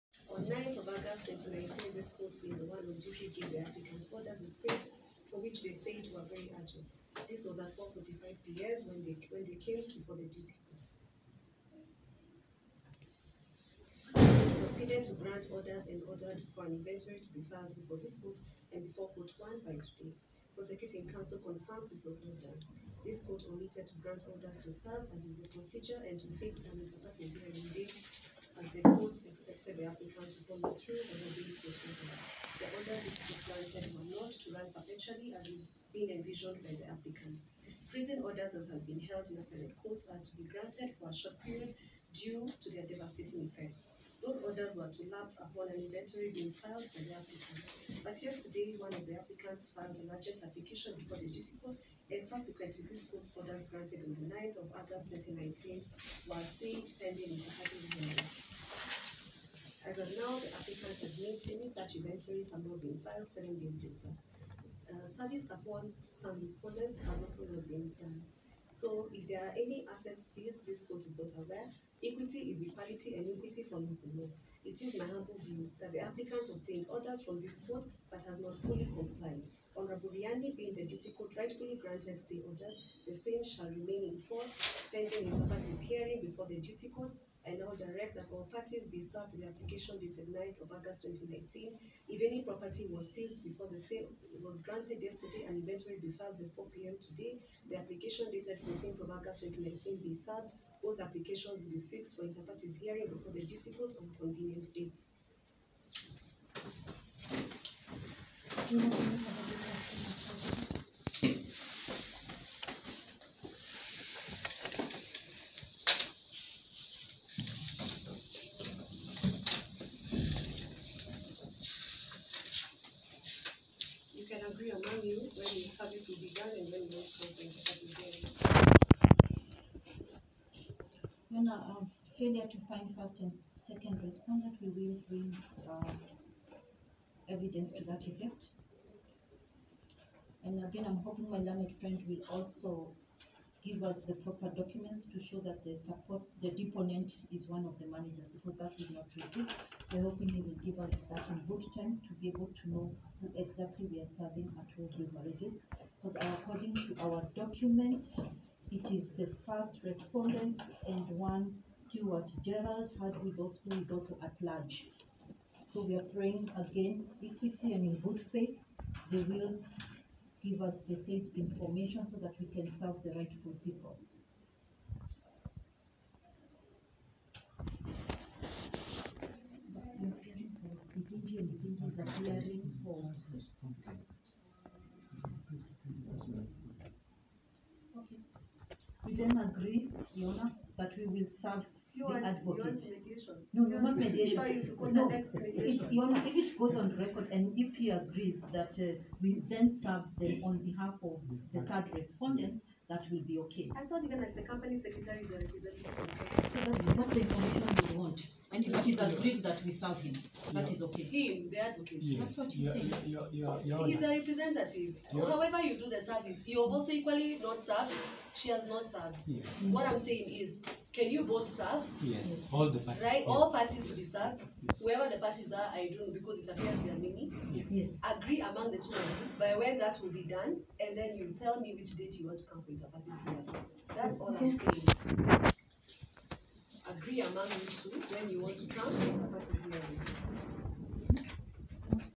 COURT LIFTS ORDER FREEZING BILLIONAIRE KARIUKI ASSETS.
A Nairobi Court has suspended orders issued for seizure of movable property for companies associated with billionaire Humphrey Kariuki, pending conclusion of investigation being carried by Directorate of Criminal Investigation.